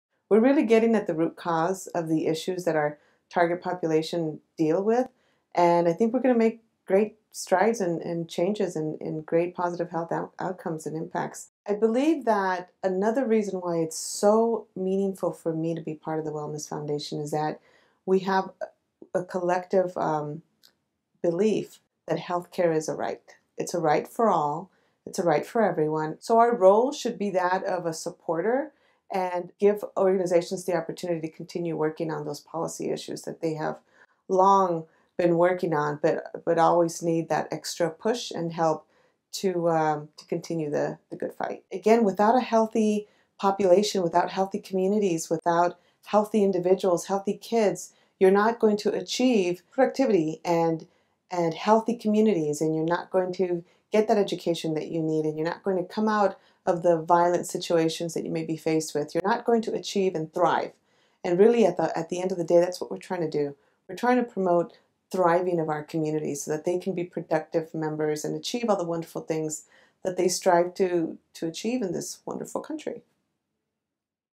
Meet Cal Wellness’ Board Vice Chair: M. Isabel Becerra
The California Wellness Foundation appointed M. Isabel Becerra as vice chair of its Board of Directors. Becerra is the chief executive officer of the Coalition of Orange County Community Health Centers, a consortium of safety net partners providing quality health care services for underserved communities. She joined the Board in January 2009.

Video by I.W. Group. Produced by The California Wellness Foundation. May 2015